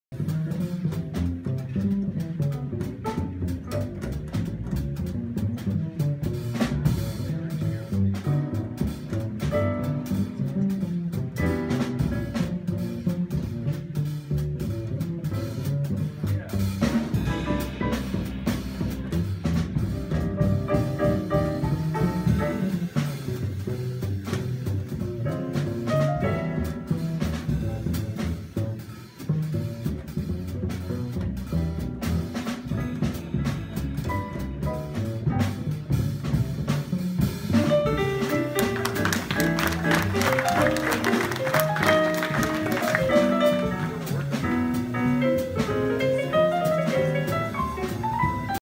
2017 Annual Jazz Scholarship Awards Concert - Ian Fuller - Bass Solo
The Space Coast Jazz Society's Annual Scholarship Awards Concert May 21, 2017 with winners Cameron Crew from Satellite High on trumpet & Ian Fuller from Viera High School on bass.